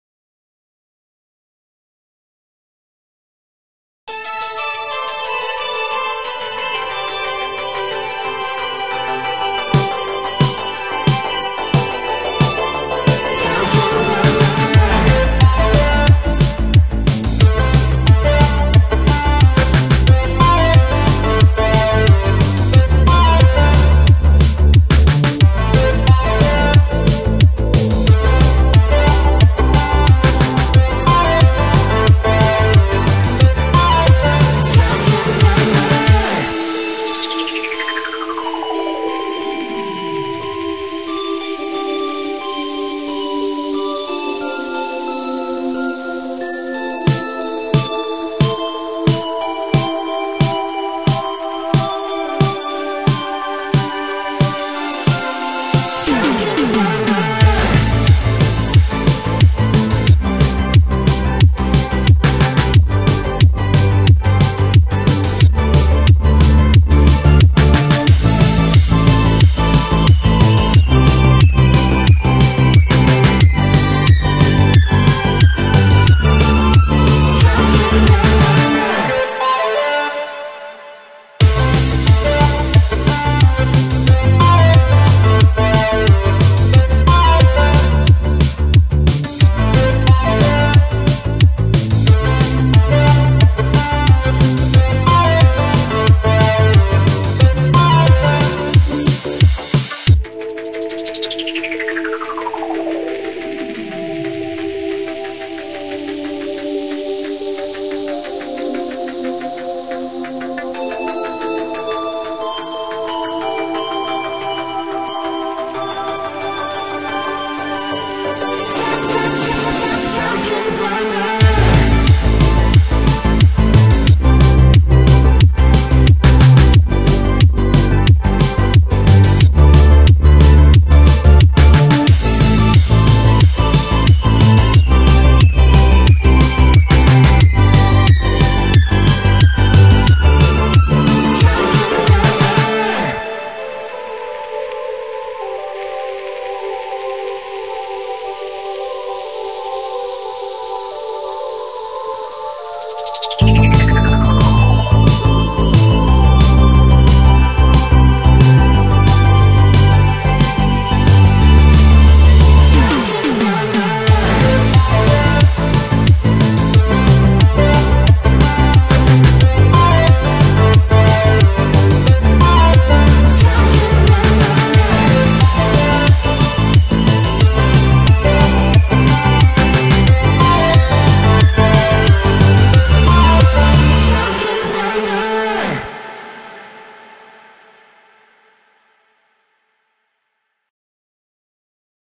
Soundscape Runner - Demo1 2021.11.04
Procedurally created level using signal detection from the music. Obstacles, coins, trees are procedurally created.

This is a work in progress.